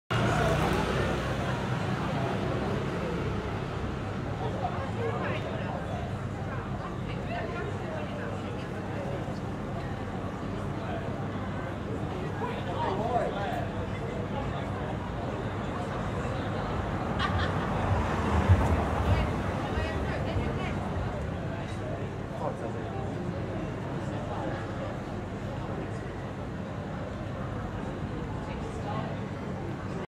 The Cricketers Pub On Black Lion Street In Brighton, UK
The Cricketers pub on Black Lion Street in Brighton, England. Wide shot followed by close up on sign.